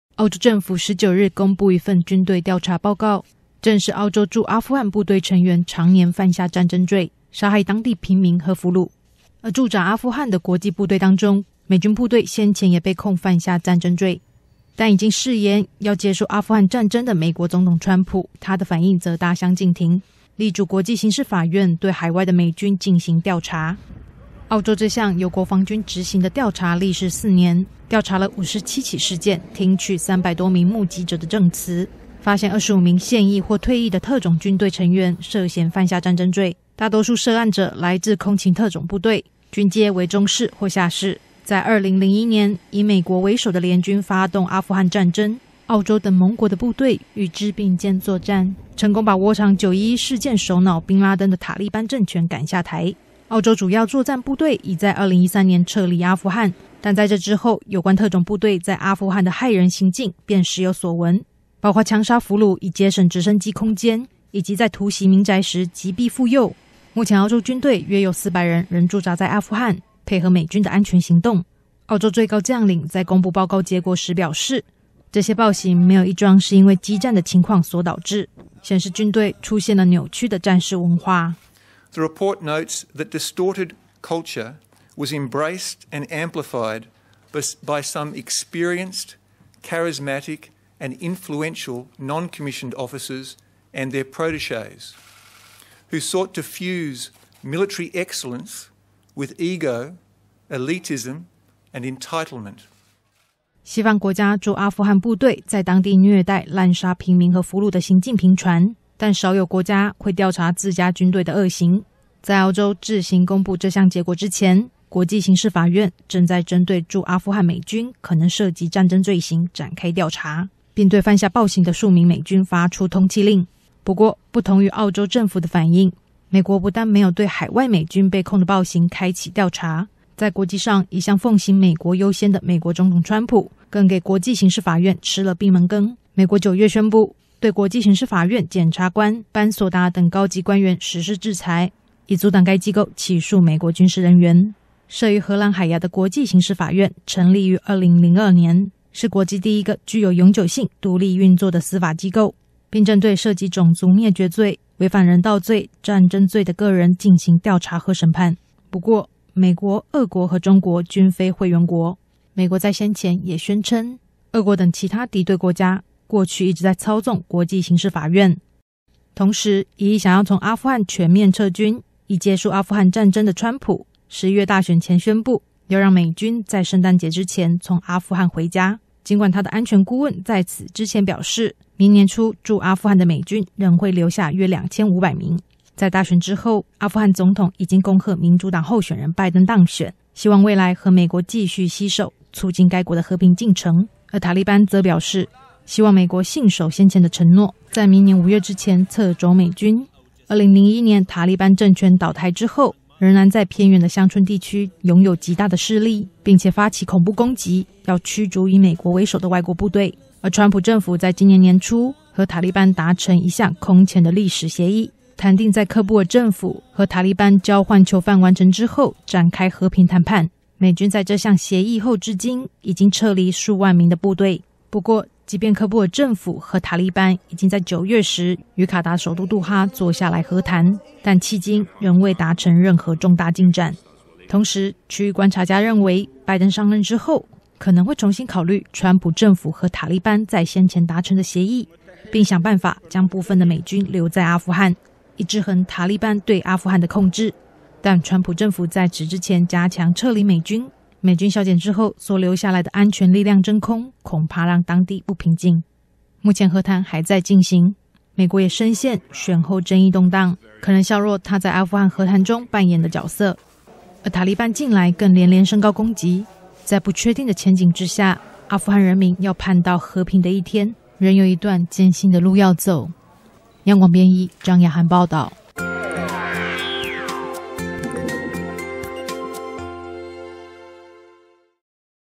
澳洲證實駐軍涉戰爭罪 阿富汗和平未卜【央廣國際新聞】
澳洲政府19日公布一份軍隊調查報告，證實澳洲駐阿富汗部隊成員長年犯下戰爭罪，殺害當地平民和俘虜。這項由澳洲國防軍(Australia Defense Force)執行的調查歷時4年，調查57起事件，聽取300多名目擊者的證詞，發現了25名現役或退役特種部隊成員涉嫌犯下戰爭罪，大多數涉案者來自空勤特種部隊(SAS)，軍階為中士與下士。